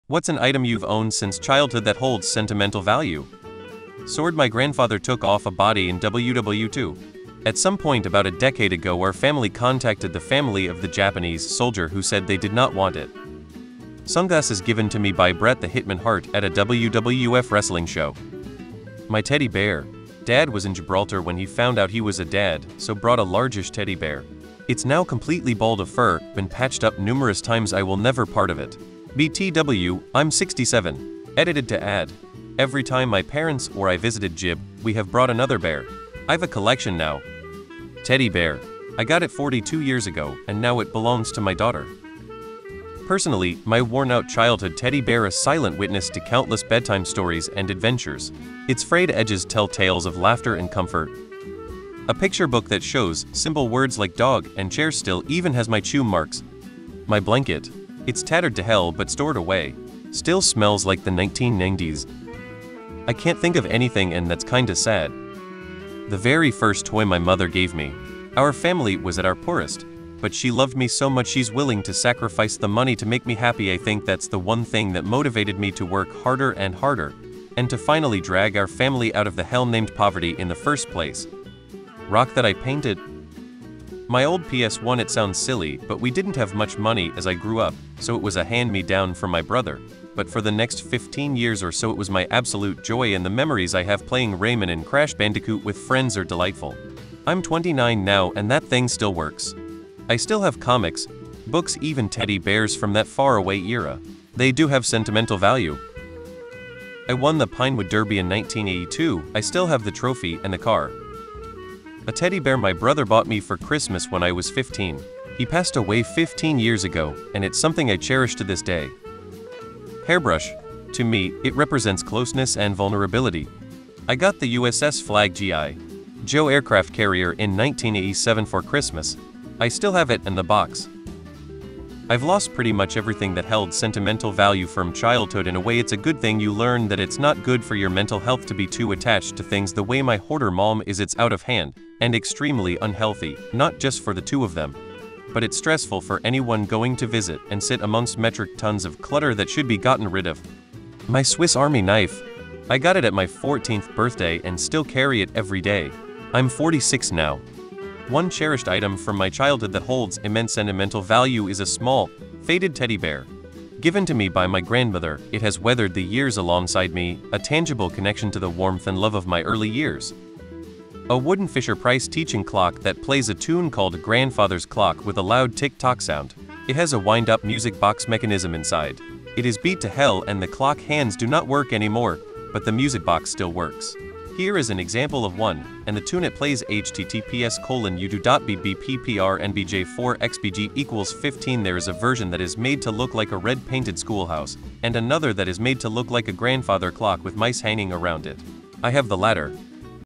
What's an item you've owned since childhood that holds sentimental value?
What's an item you've owned since childhood that holds sentimental value? the question you all have been asking for. I have been asked this question so many times that I decided to make a video about it. I hope you enjoy it and if you have any questions, please leave them in the comments below.